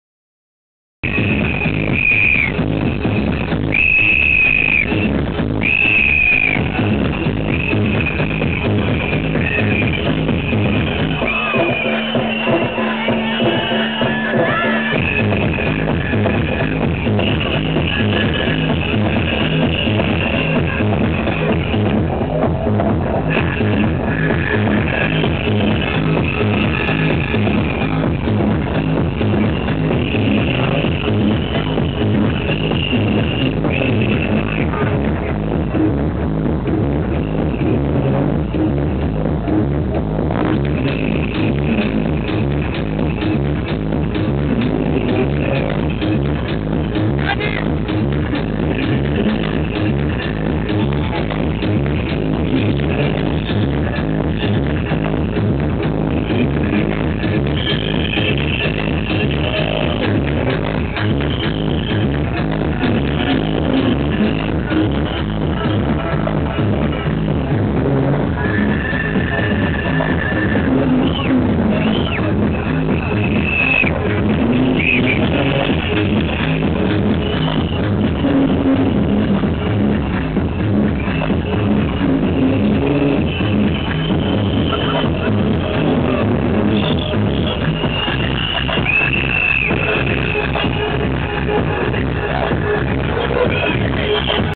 KARNAVALI 2009 RETHYMNO PARTY ALLADALLON vol.6
After the Carnival pare in Rethymno Greece, there was a street party from the Carnival team "ALLADALLON" outside from the museum in the old town (sponsors also from other teams "Ypoloipoi group" "Rodolinoi", "Alter Ego", "Arkaloi", "Daktilodiktoymenoi" and "Alloi"